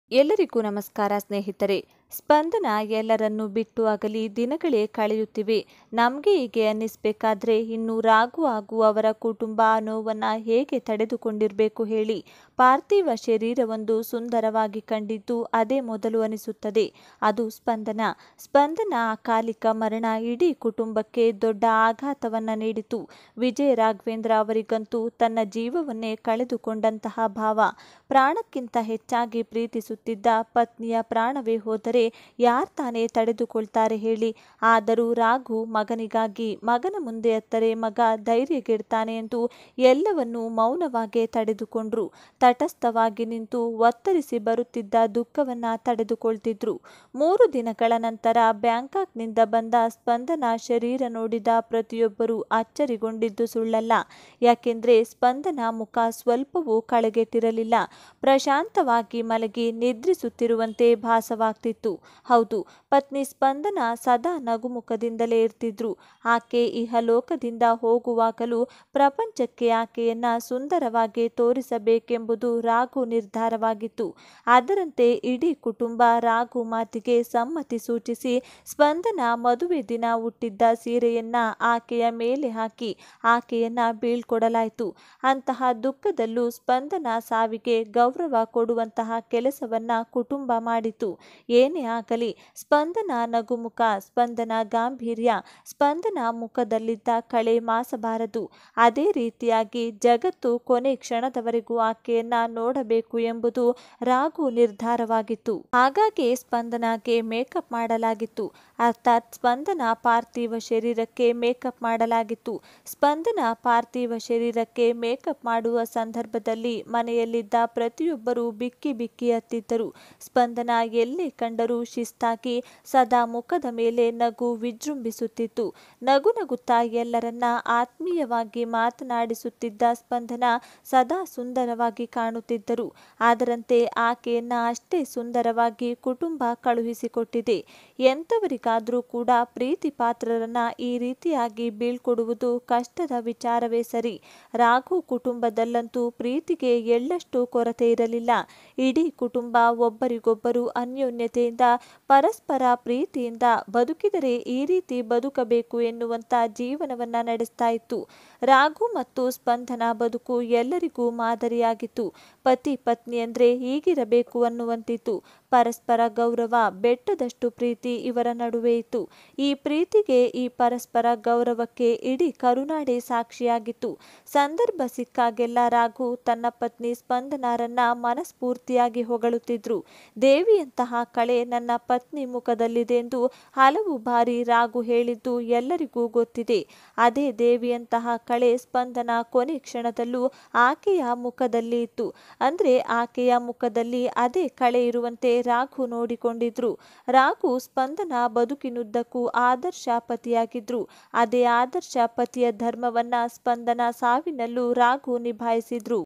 ಸ್ಪಂದನ ಗೆ ಕೊನೆಗೆ ಮೇಕಪ್ ಮಾಡಿದ್ದು ಯಾಕೆ ? ಯಾರು? - ಅಳುತ್ತಾ ರಾಘು ಹೇಳಿದ್ದೇನು? | SpandanaVijay
ಸ್ಪಂದನ ಗೆ ಕೊನೆಗೆ ಮೇಕಪ್ ಮಾಡಿದ್ದು ಯಾಕೆ ? ಯಾರು? - ಅಳುತ್ತಾ ರಾಘು ಹೇಳಿದ್ದೇನು? | SpandanaVijay | Kannada Thare Tv